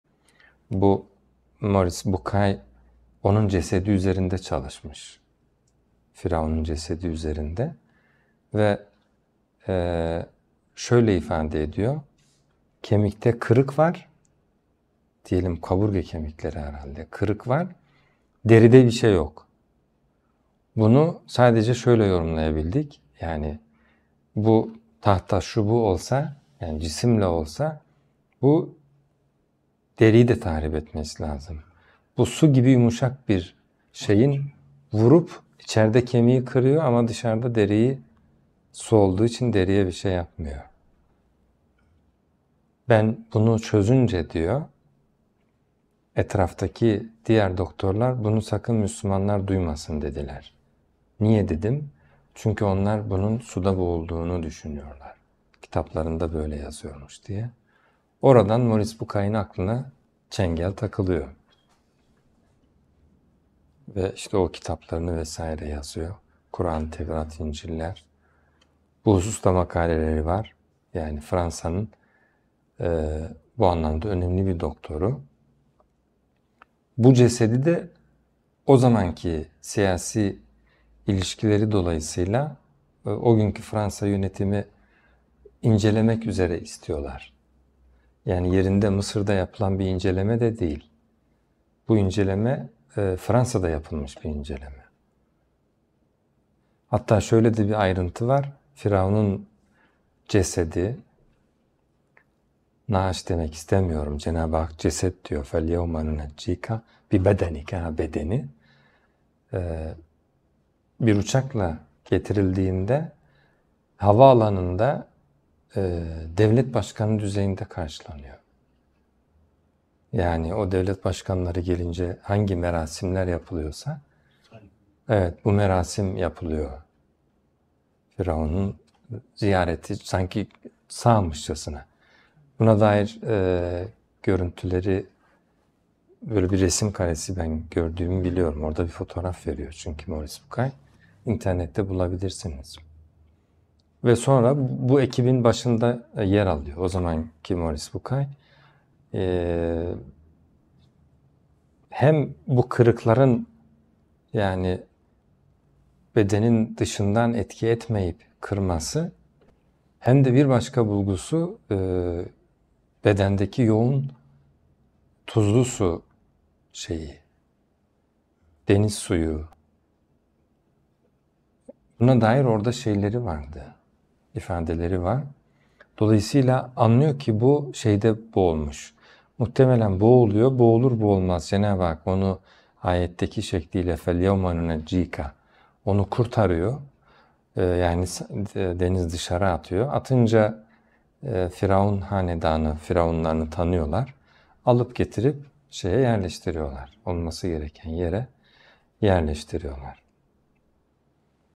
Dr. Maurice Bucaille Firavun Cesedi Üzerindeki İncelemesi
A'raf 136 | Meal 2. Ders | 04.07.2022
👆 Dersinden alıntılanmıştır.

✅DERSİN TAMAMI İÇİN

✔️Prof. Dr. Halis AYDEMİR

HECE KİTAP VE HİKMET DERNEĞİ - BURSA
Küplüpınar, Dr. Sadık Ahmet Cd. No: 328, 16200 Osmangazi/Bursa

Derslerin güncel takvimine sitemizden ulaşabilirsiniz